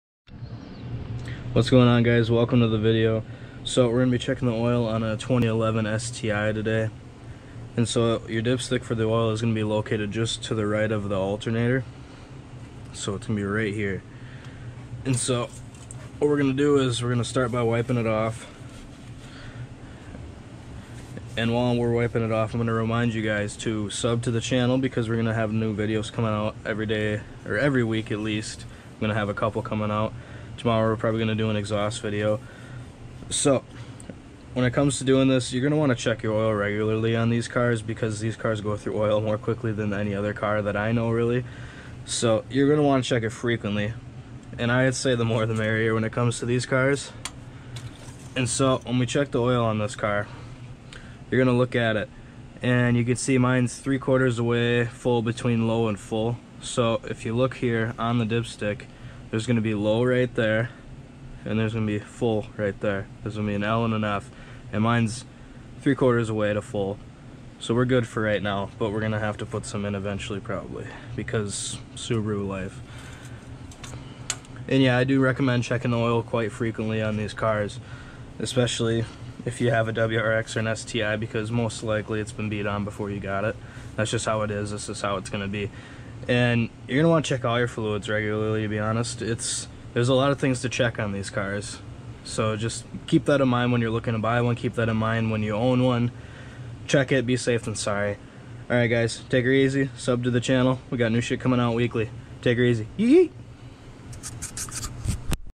How to check oil Subaru WRX STI (EASY)
How to check your oil on a 2011 Subaru Impreza WRX STI 2.5L Turbo Charged Boxer Engine.

Not Liable for anything that happens to anyone's vehicle.